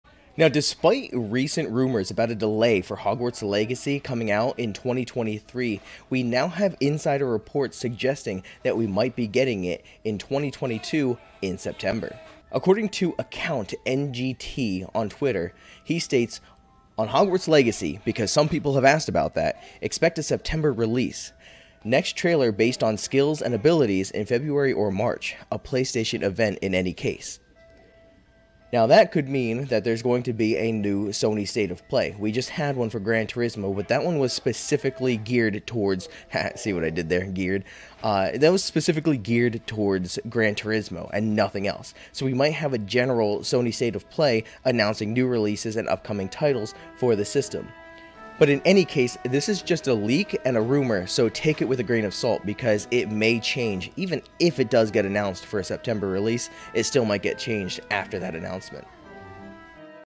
Are you excited for the release of Hogwarts Legacy??
Hogwarts Legacy has long been rumored to be delayed until 2023. New rumors and inside information suggest otherwise. Are you excited for the release of HogwartsLegacy ?? harrypotter wizardingworld fantasticbeasts TeamofTomorrow